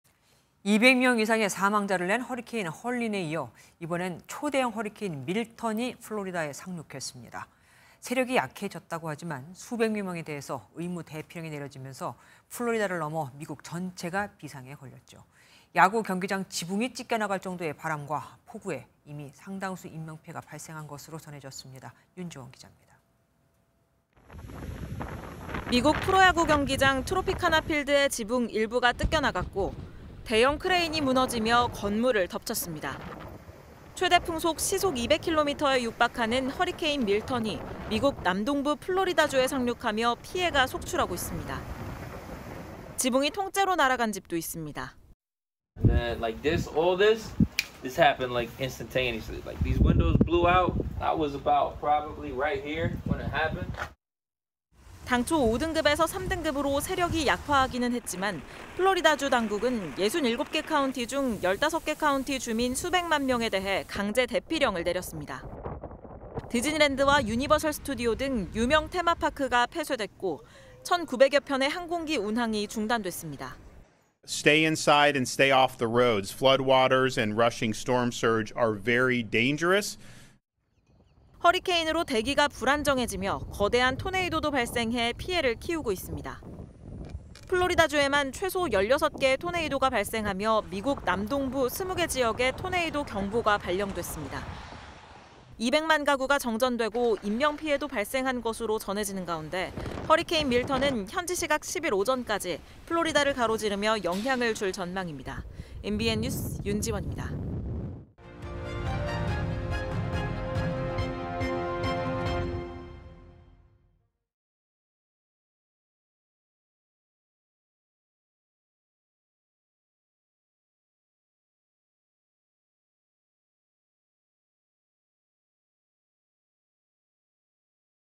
허리케인 '밀턴' 상륙에 플로리다 '비상'…수백만에 대피령 [MBN 뉴스7]
【 앵커멘트 】
  200명 이상의 사망자를 낸 허리케인 헐린에 이어 초대형 허리케인 밀턴이 2주 만에 다시 플로리다에 상륙했습니다.
  세력이 약해졌다지만, 수백만 명에 대해 의무 대피령이 내려지면서 플로리다를 넘어 미국 전체가 비상에 걸렸습니다.
  벌써 상당수 인명피해가 발생한 것으로 전해집니다.
  윤지원 기자입니다.

【 기자 】
  미국 프로야구 경기장 트로피카나 필드의 지붕 일부가 뜯겨 나갔고, 대형 크레인이 무너지며 건물을 덮쳤습니다.

  최대 풍속 시속 200km에 육박하는 허리케인 '밀턴'이 미국 남동부 플로리다주에 상륙하며 피해가 속출하고 있습니다.

  지붕이 통째로 날아간 집도 있습니다.

▶ 인터뷰 : 코너 페린 / 파손된 주택 소유주
- "이 모든 일이 창문이 날아가는 것처럼 순식간에 일어났어요. 저는 일이 벌어졌을 때 바로 여기 있었던 것 같아요."

  당초 5등급에서 3등급으로 세력이 약화하기는 했지만, 플로리다주 당국은 67개 카운티 중 15개 카운티 주민 수백만 명에 대해 강제 대피령을 내렸습니다.

  디즈니 랜드와 유니버설 스튜디오 등 유명 테마파크가 폐쇄됐고, 1,900여 편의 항공기 운항이 중단됐습니다.

▶ 인터뷰 : 론 드산티스 / 미국 플로리다주 주지사
- "집 안에 머물고 도로에 나가지 마십시오. 홍수와 밀려오는 폭풍 해일은 매우 위험합니다."

  허리케인으로 대기가 불안정해지며 거대한 토네이도도 발생해 피해를 키우고 있습니다.

  플로리다주에서만 최소 16개의 토네이도가 발생하며 미국 남동부 20개 지역에 토네이도 경보가 발령됐습니다.

  200만 가구가 정전되고 인명피해도 발생한 것으로 전해지는 가운데, 허리케인 밀턴은 현지시각 10일 오전까지 플로리다를 가로지르며 영향을 줄 전망입니다.

  MBN뉴스 윤지원입니다.